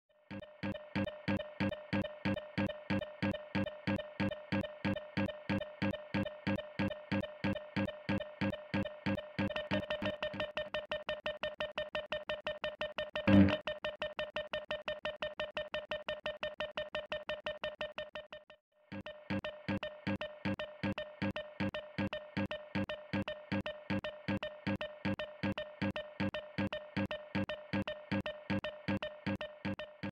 O Perdedor (Loser)
Here's a famous story seen by a different point of view: the ghost's.
This is supposed to be played in a loop.
Animação finalista do Anima Mundi Celular 2008.